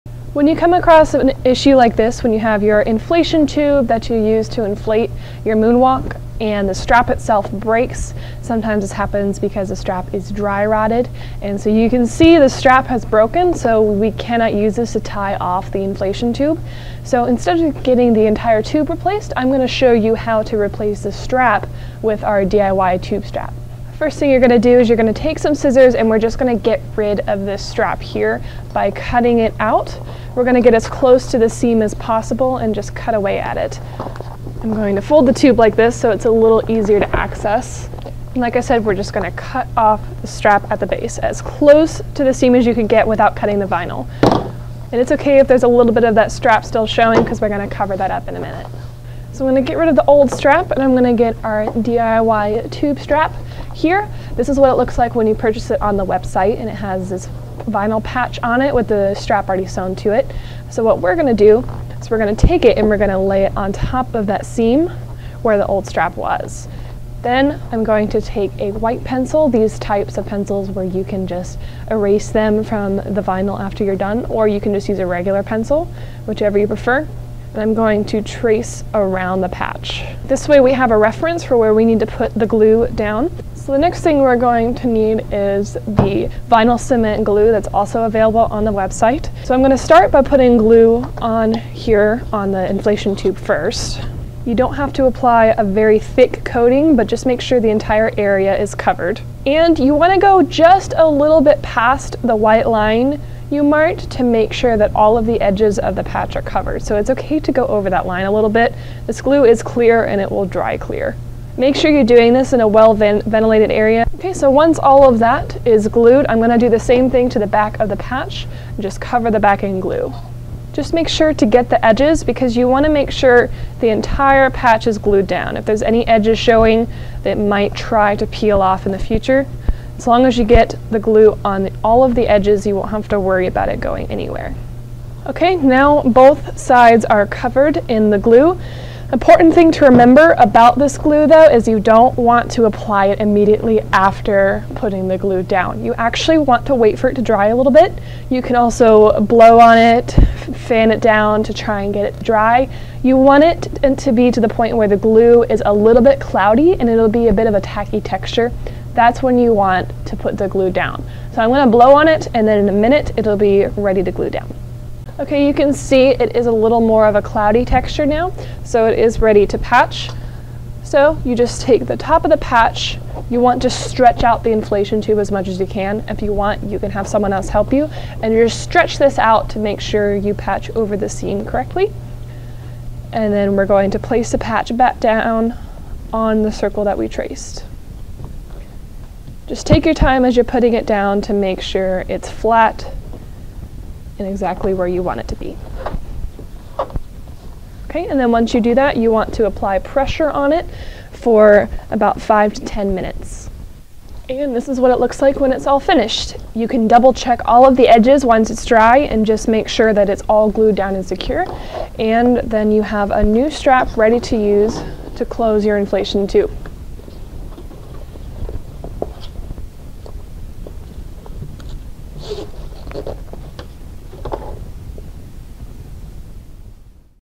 How to install a DIY inflation tube strap - Bounce House Tutorial
For more information on all of our DIY repair accessories, visit our

Transcription:
When you come across an issue like this when you have your inflation tube that you use to inflate your moonwalk and the strap itself breaks, sometimes this happens because the strap is dry rotted, and so you can see the strap is broken so we cannot use this to tie off the inflation tube. So instead of getting the entire tube replaced, I'm going to show you how to replace this strap with our DIY tube strap. First thing you're going to do is you're going to take some scissors and we're just going to get rid of this strap here by cutting it out. We're going to get as close to the seam as possible and just cut away at it. I'm going to fold the tube like this so it's a little easier to access. Like I said, we're just going to cut off the strap at the base as close to the seam as you can get without cutting the vinyl. And it's ok if there's a little bit of that strap still showing because we're going to cover that up in a minute. So let me get rid of the old strap and I'm going to get our DIY tube strap here. So what we're going to do, is we're going to take it and lay it on top of that seam where the old strap was. Then, I'm going to take a white pencil-these types of pencils where you can just erase them from the vinyl after you're done, or you can just use a regular pencil, whichever you prefer-and I'm going to erase around the patch. This way we have a reference for where we need to put the glue down. So the next thing we're going to need is the vinyl cement glue that's also available on the website. So I'm going to start by putting glue on here on the inflation tube first. You don't have to apply a very thick coating, but just make sure the entire area is covered. And you want to go just a little bit past the white line you marked to make sure that all of the edges of the patch are covered. So it's ok to go over that line a little bit. This glue is clear and it will dry clear. Make sure you're doing this in a well ventilated area. Ok so once all of that is glued, I'm going to do the same thing to the back of the patch. Just cover the back in glue. Just make sure to get the edges. Because you want to make sure the entire patch is glued down. If there's any edges showing, it might try to peel off in the future. As long as you get the glue on all of the edges, you won't have to worry about it going anywhere. Ok, now both sides are covered in the glue. An important thing to remember about this glue, though, is you don't want to apply it immediately after putting the glue down. You actually want to wait for it to dry a little bit. You can also blow on it, fan it down, to try and get it to dry. You want it to be to the point where the glue is a little bit cloudy and it'll be a little bit of a tacky texture. That's when you want to put the glue down. So I'm going to blow on it, and in a minute it'll be ready to glue down. Ok you can see it is a little more of a cloudy texture now, so it is ready to patch. So, you just take the top of the patch, you want to stretch out the inflation tube as much as you can, and if you want you can have someone else help you, and you'll stretch this out to make sure you patch over the seam correctly. And then we're going to place the patch back down on the circle that we traced. Just take your time as your time as you're putting it down to make sure it's flat and exactly where you want it to be. Ok, and then once you do that you want to apply pressure on it for about five to ten minutes. And this is what it looks like when it's all finished. You can double check all of the edges once it's dry and just make sure that it's all glued down and secure. And then you have a new strap ready to use to close your inflation tube.